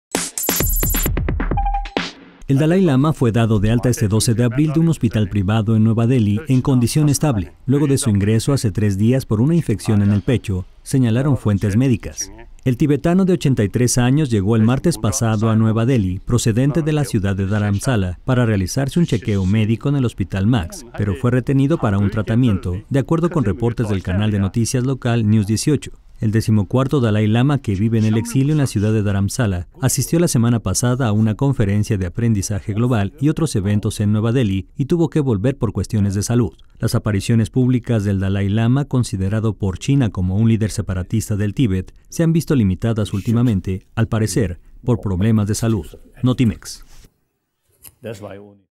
Dalai Lama abandona hospital en condición estable
El Dalai Lama fue hoy dado de alta de un hospital privado en Nueva Delhi en condición estable, luego de su ingreso hace tres días por una infección en el pecho, señalaron fuentes médicas.